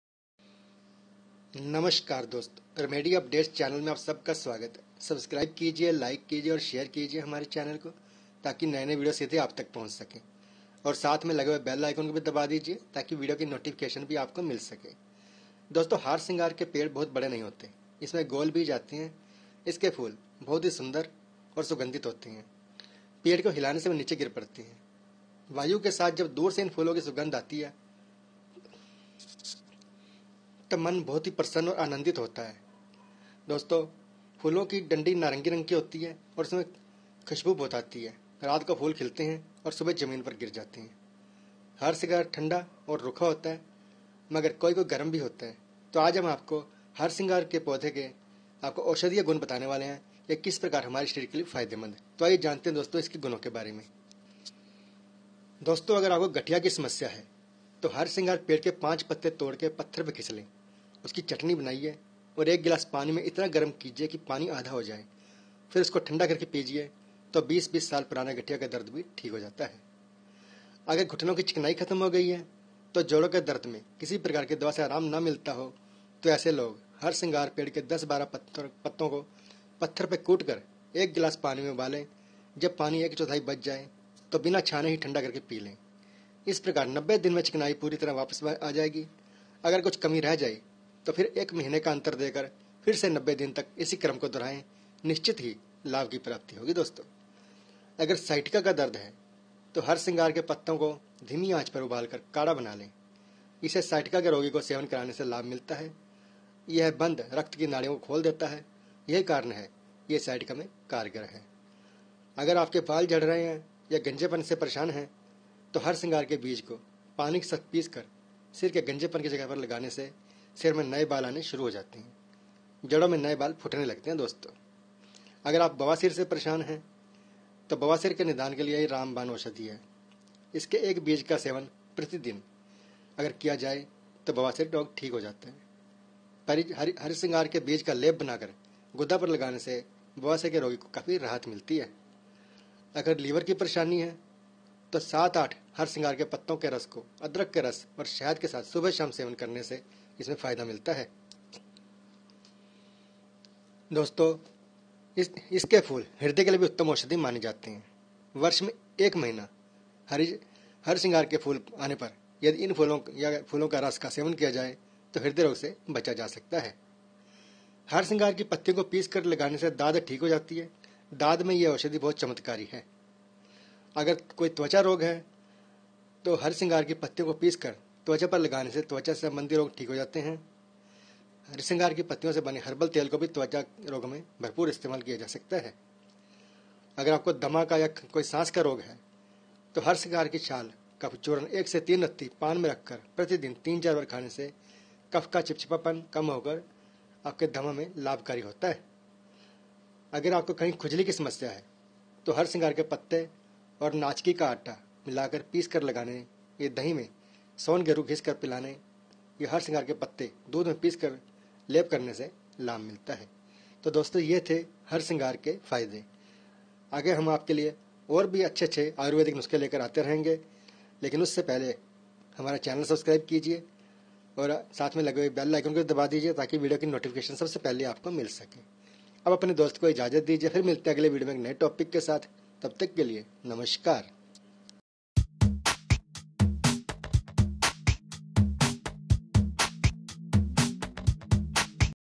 यह पौधा जीवनभर नही होने देगा गठिया सायटिका,गंजापन और नसों के ब्लॉकेज आदि 14 रोगों को, ऐसे करे प्रयोग
यह पौधा जीवन भर नही होने देगा गठिया, सायटिका, जोड़ों का दर्द, गंजापन और नसों के ब्लॉकेज आदि 14 रोगों को, ऐसे करे प्रयोग